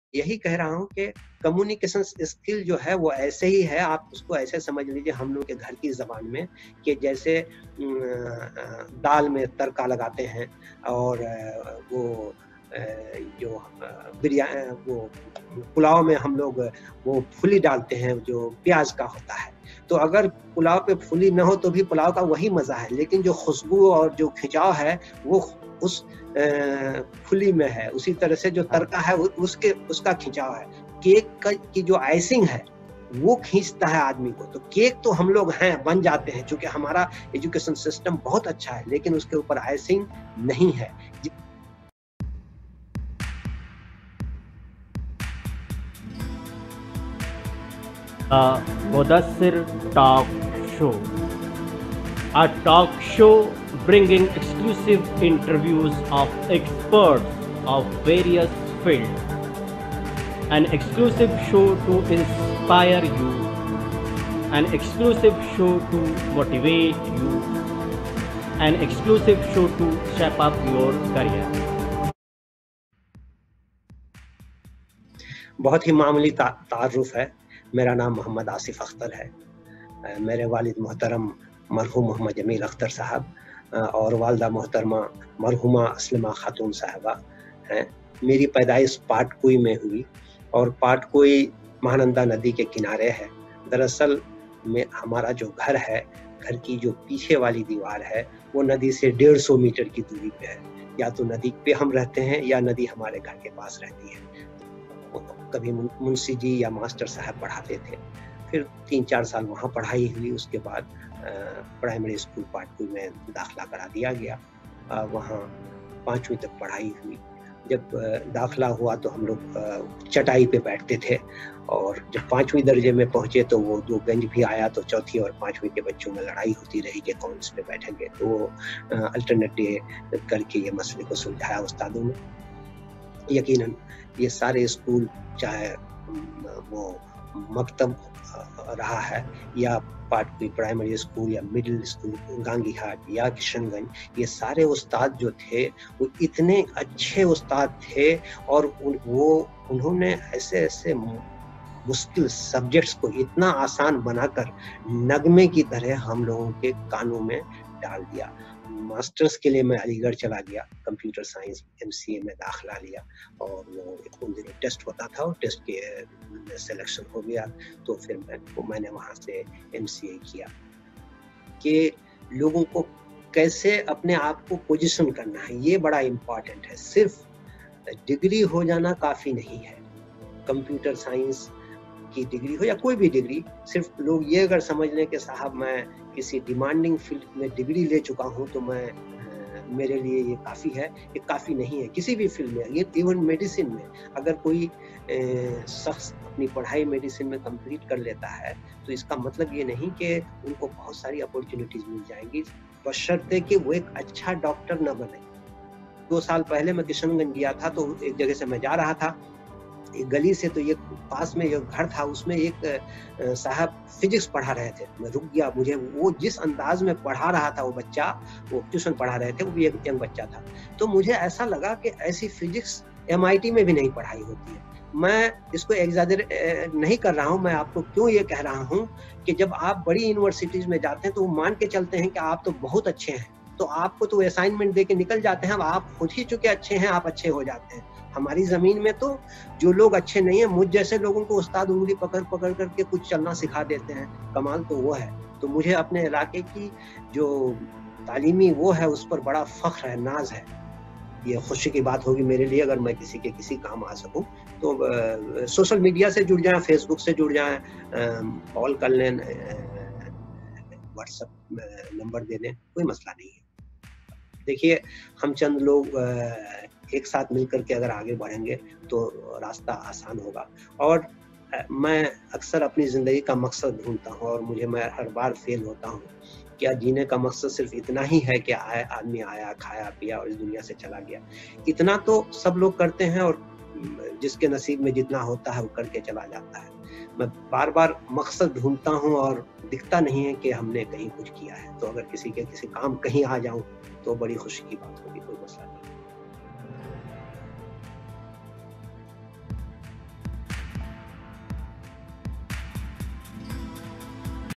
TMTS Episode 5 - Teaser of Interview with Mr. Asif Akhter, Sr. IT Consultant, Govt. of Saudi Arabia
Exclusive Interview with Mr. Asif Akhter, a resident of Kishanganj, a MCA graduate from Aligarh Muslim University and currently IT Consultant in an oil refinery company of Government of Saudi Arabia.